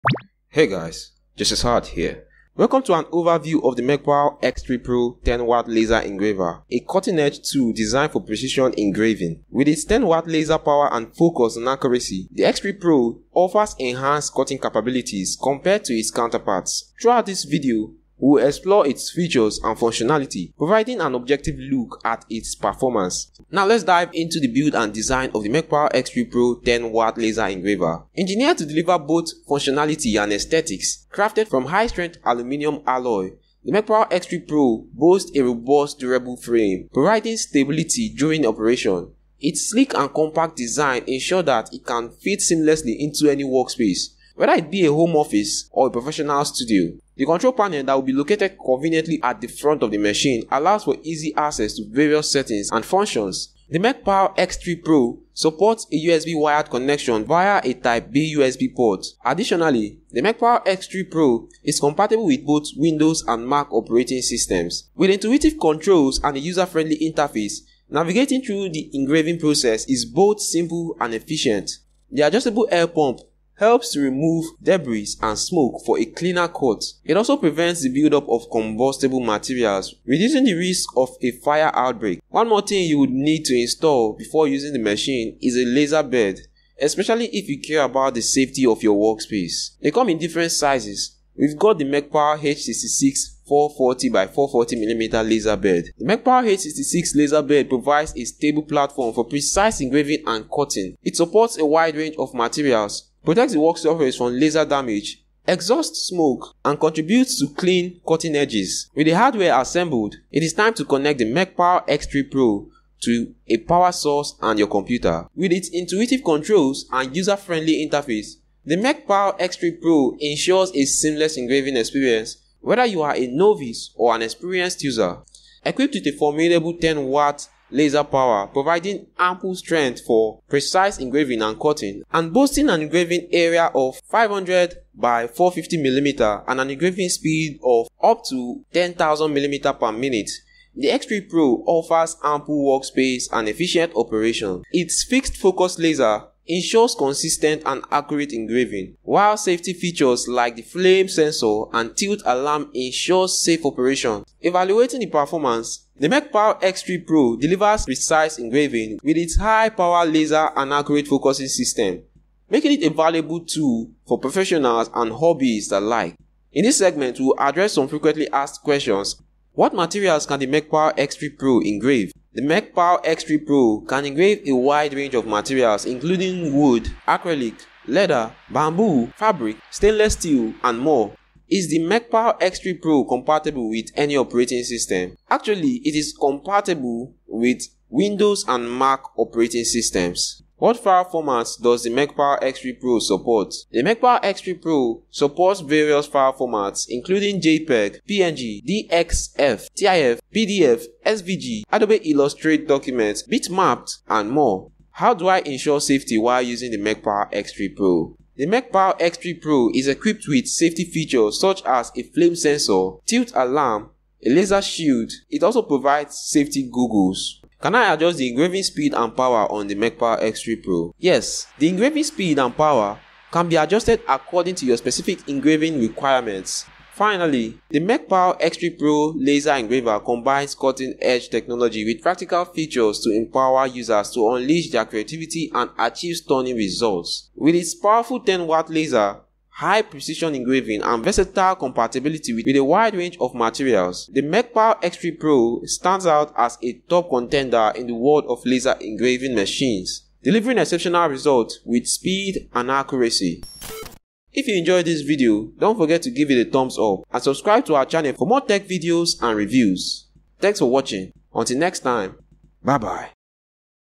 Mecpow X3 Pro Laser Engraver Overview and Quick Setup Guide
the Mecpow X3 Pro 10W Laser Engraver machine lets you make permanent prints on cardboard, wood, glass, textiles, paper, and plastic materials.

In this Mecpow X3 Pro Overview video, we will guide you on how to set up the Mecpow X3 Pro 10W Laser Engraver as well as highlight its features, specifications, and performance capabilities.